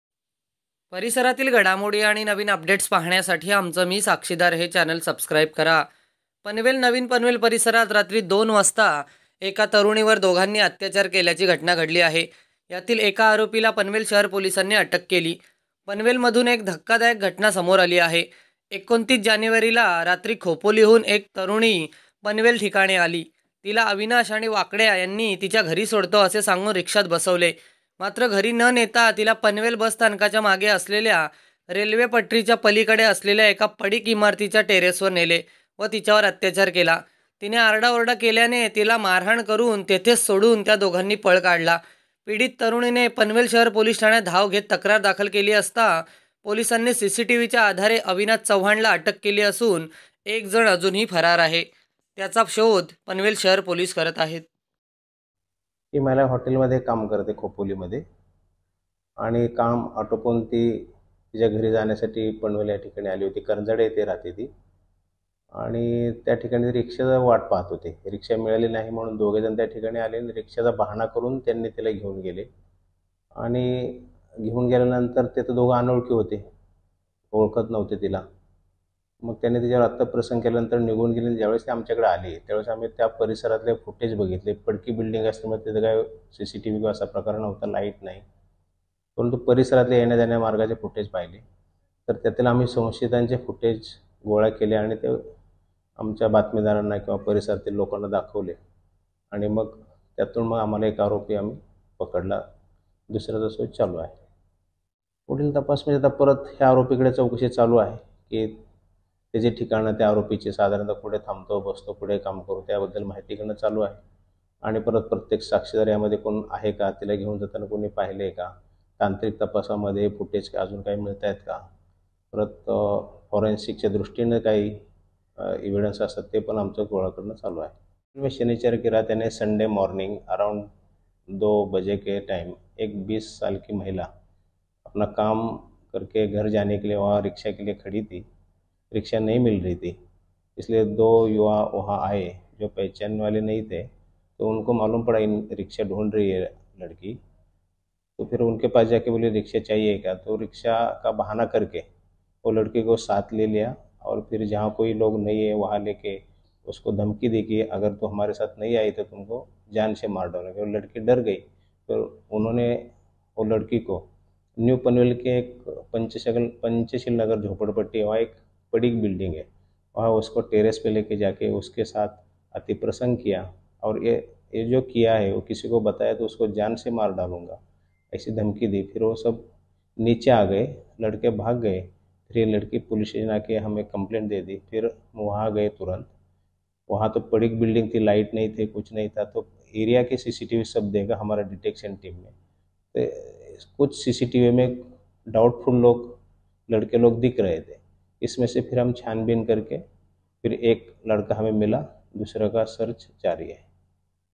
पनवेलमध्ये दोघांनी केले नको ते कृत्य, गुन्हा दाखल
पनवेलमध्ये रात्रीच्या सुमारास तरुणीवर केला अत्याचार,तरुणीला पडक्या इमारतीत नेतानाचा व्हिडिओ आला समोर